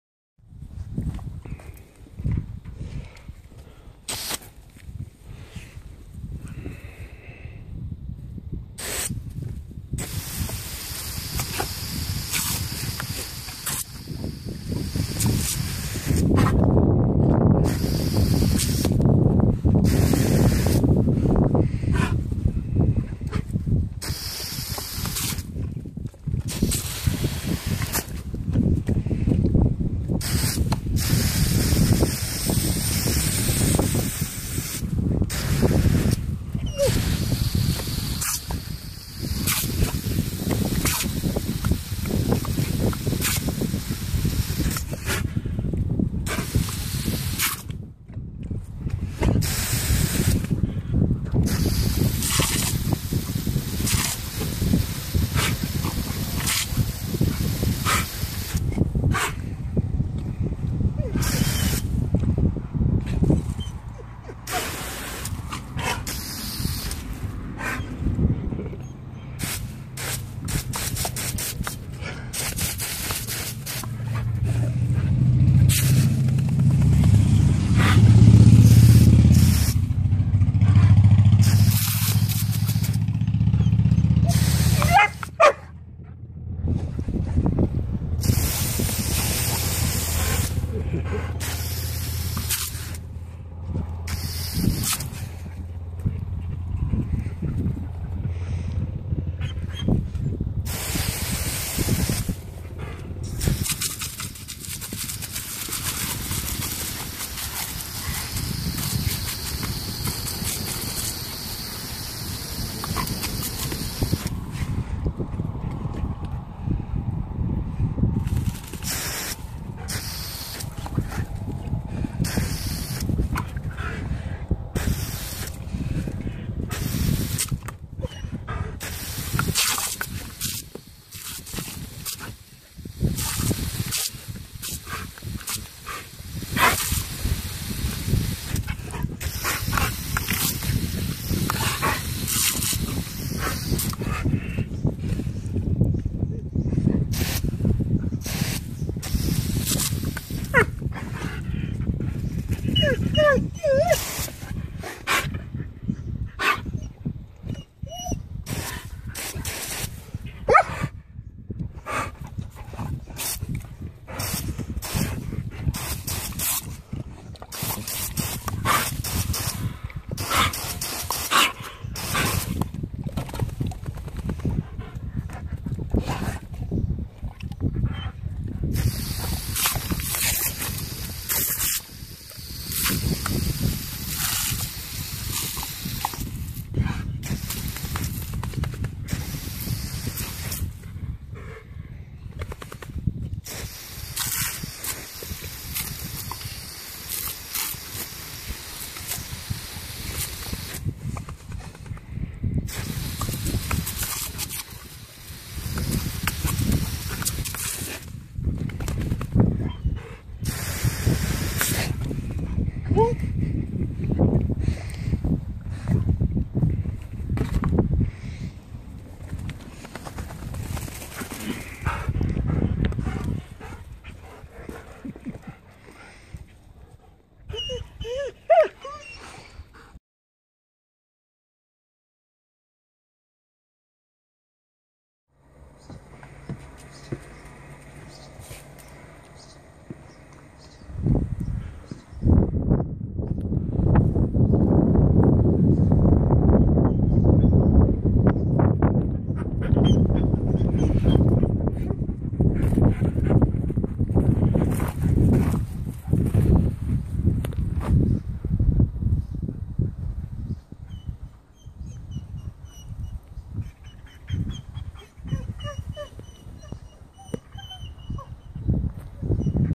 Cooling off my dogs in the summer heat
Not the usual game-stuff this time.
Just wanted to share with you some fun my dogs and I had this summer.